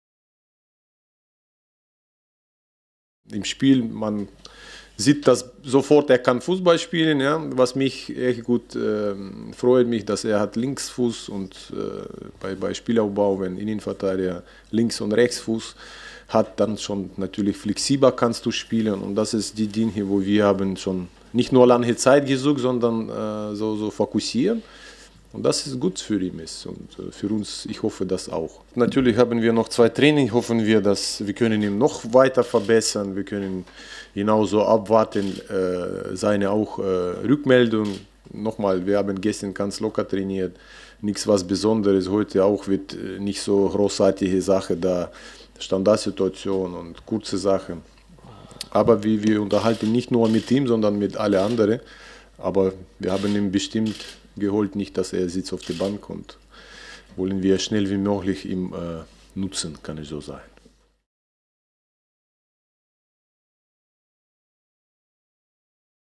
Skripniks erste Eindrücke von Djilobodji
Bremen, 22.01.16: Seit Donnerstag trainiert Neuzugang "Papy" Djilobodji mit seinen neuen Kollegen von Werder Bremen. Der Innenverteidiger kann beidfüßig spielen, das freut Trainer Viktor Skripnik. Der Senegalese kommt auf Leihbasis vom FC Chelsea bis zum Saisonende für den nach China abgewanderten Assani Lukimya.